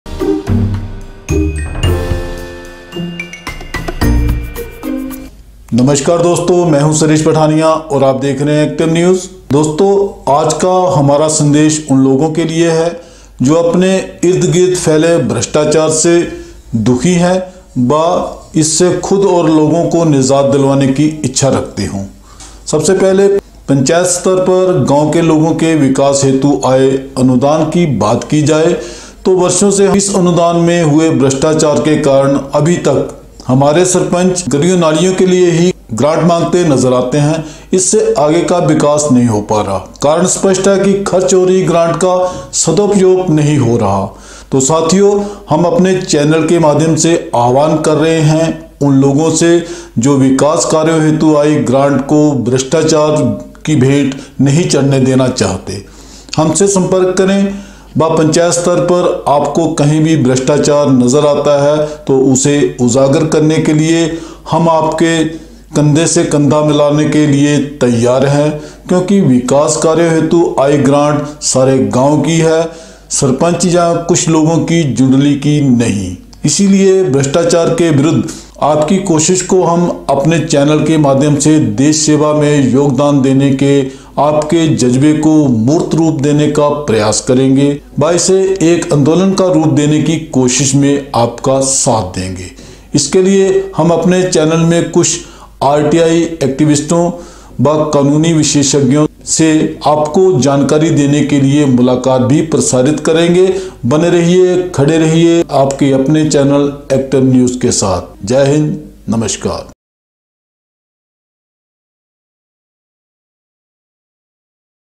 आओ हाथ बढ़ाएं भ्रष्टाचार भगाएं व् लोकधन बचाएं
दोस्तों आज का हमारा सन्देश उन लोगों के लिए है जो अपने इर्द-गिर्द फैले भ्रष्टाचार से दुखी हैं व् इससे खुद व् लोगों को निजात दिलाने की इच्छा रखते हों सबसे पहले पंचायत स्तर पर गाँव के लोगों के विकास हेतु आए अनुदान की बात की जाये तो वर्षों से हम इस अनुदान में हुए भ्रष्टाचार के कारण अभी तक हमारे सरपंच गलियों नालियों की ग्रांटके लिए ही ग्रांट मांगते नज़र आते हैं इससे आगे विकास हो ही नहीं पा रहा कारण स्पष्ट है के खर्च हो रही ग्रांट का सदोपयोग नहीं हो रहा तो साथियो हम अपने चेनल के माध्यम से आह्वान कर रहे हैं उन लोगों से जो विकास कार्यों के हेतु आई ग्रांट को भ्रष्टाचार की भेंट नहीं चढ़ने देना चाहते हमसे सम्पर्क करें व् पंचायत स्तर पर आपको कहीं भ्र्ष्टाचार नज़र आता है तो उसे उजागर करने के लिए हम आपके कंधे से कन्धा मिलाने के लिए तैयार हैं क्योंकि विकास कार्यों हेतु आई ग्रांट सारे गाँव की है सरपंच या कुछ लोगों की जुंडली की नहीं इसलिए भ्रष्टाचार के विरुद्ध आपकी कोशिश को हम अपने चैनल के माध्यम से देश सेवा में योगदान देने के आपके ज़ज़्बे को मूर्त रूप देने का प्रयास करेंगे व् इसे एक आंदोलन का रूप देने की कोशिश में आपका साथ देंगे इसके लिए हम अपने चेनल में कुछ आर.टी.आई .एक्टिविस्टों,व् कानूनी विशेषज्ञों से आपको जानकारी देने के लिए मुलाक़ात भी प्रसारित करेंगे बने रहिये खड़े रहिये आपके अपने चेनल एक्टिव न्यूज़ के साथ                                                                                                          '